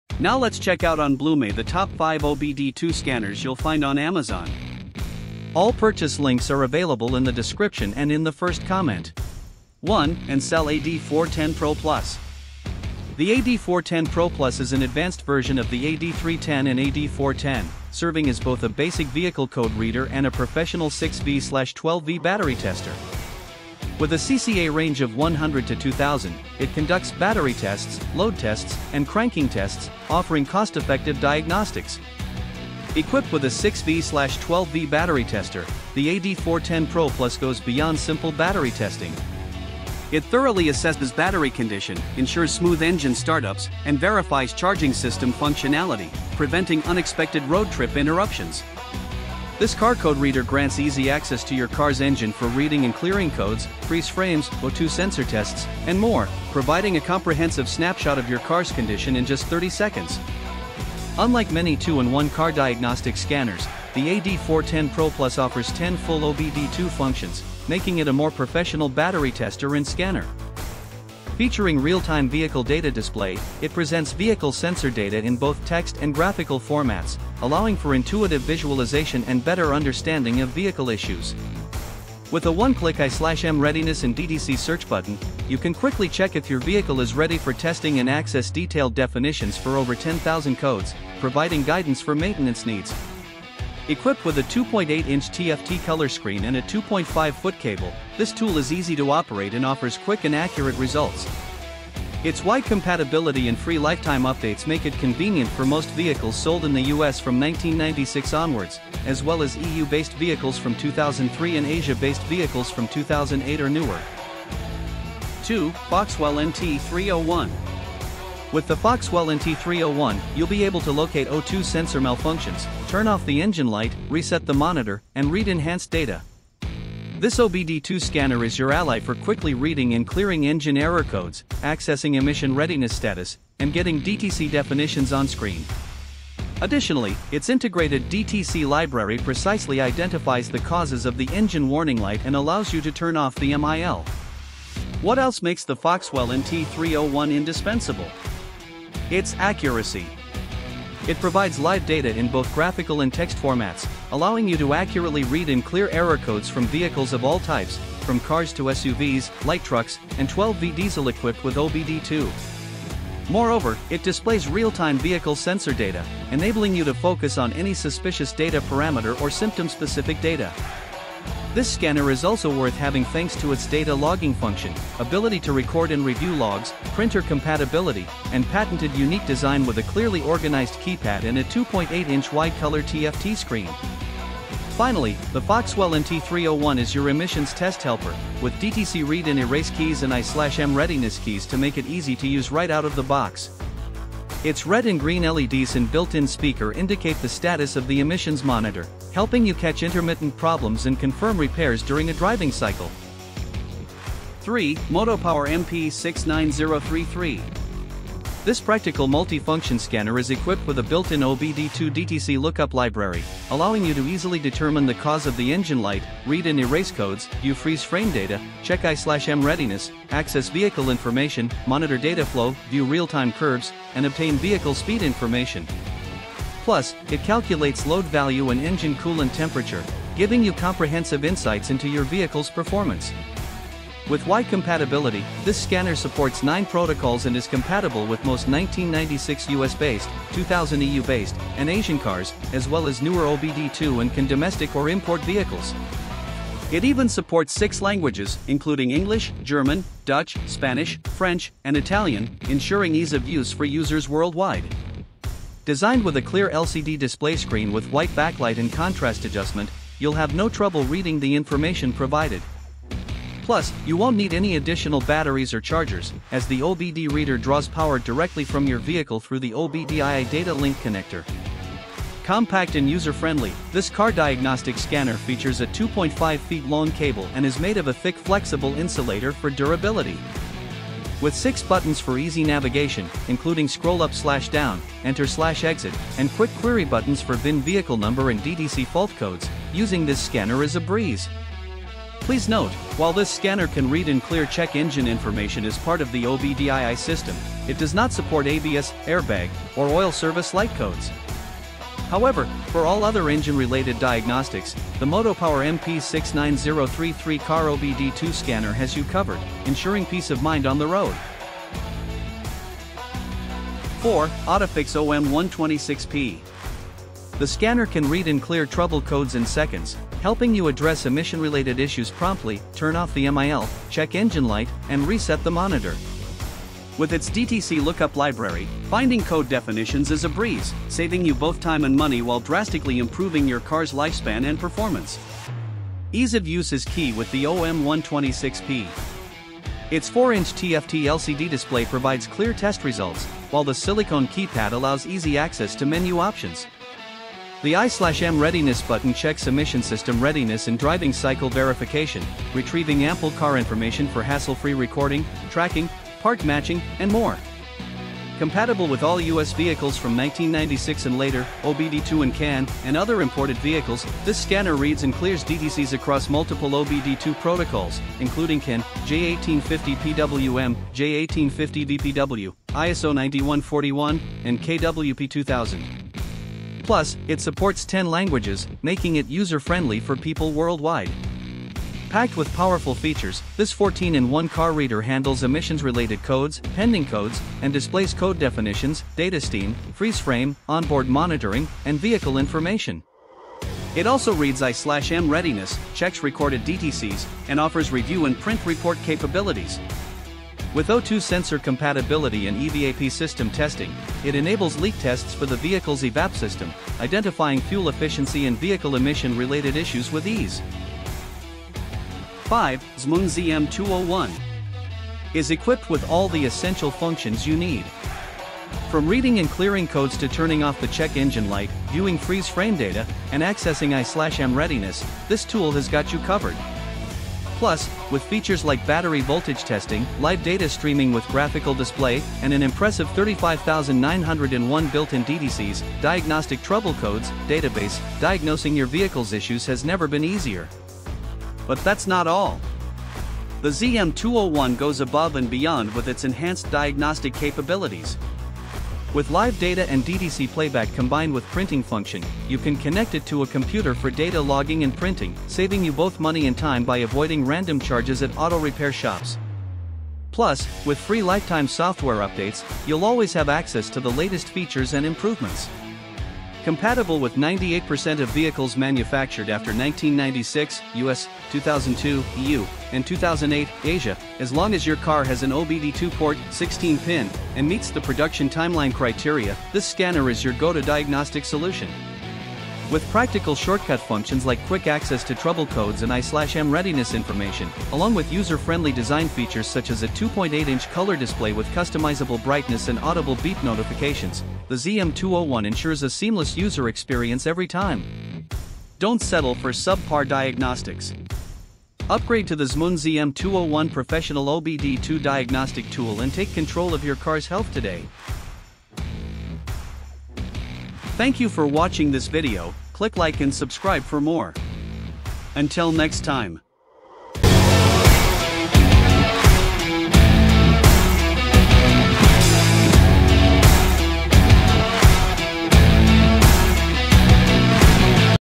🟡 🚙 Top 5 Best OBD2 Scanners Diagnostic Tool on amazon 🚗 for your car Automotive diagnostic tools
OBD2 scanners are automotive diagnostic tools designed to connect to a vehicle's onboard systems and detect operational issues.
👉 Purchase your product here

🔹 1. Ancel AD410 Pro+

🔹 2. FoXWELL NT301

🔹 3. Motopower MP69033

🔹 4. Autophix OM126P

🔹 5. Zmoon ZM201

They utilize the OBD2 (On-Board Diagnostics II) standard to access information stored in the car's computer.

These scanners allow users to read error codes, check emission test readiness status, access real-time vehicle data, and clear fault codes.

They also offer advanced functions such as the ability to turn off the check engine light (MIL), view freeze frame data, perform component tests, and more.

The benefits of OBD2 scanners include the ability to quickly and accurately diagnose mechanical and electrical problems, which can help save time and money on unnecessary repairs.

They also provide vehicle owners with a better understanding of their car's health and performance, enabling them to make informed decisions about necessary maintenance and repairs.